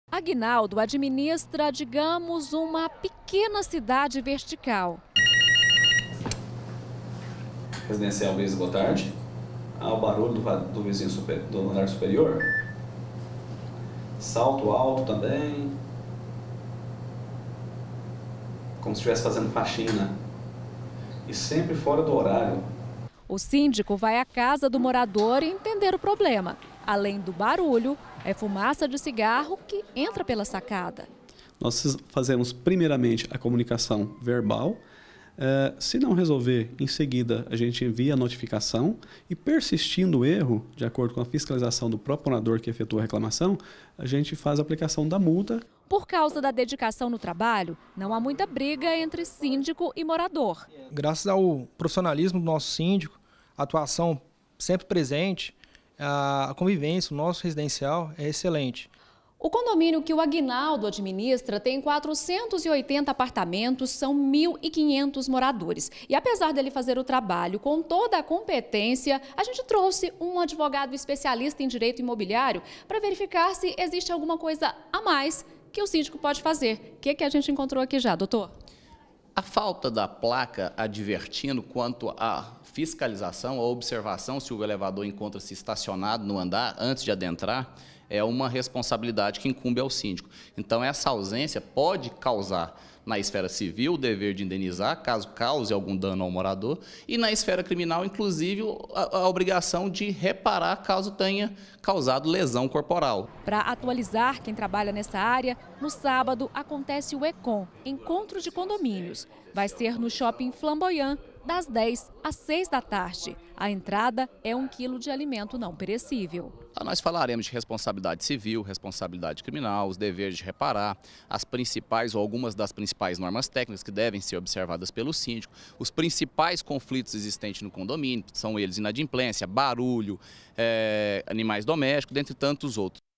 GR - Síndico: cargo exige dedicação - 12-09-2018
O síndico é a pessoa mais importante para o bom funcionamento de um condomínio. Sem ele, é caos na certa. Mas a verdade é que nem todo síndico tem conhecimento suficiente ou está preparado para o cargo. Não basta ser bem relacionado com os vizinhos!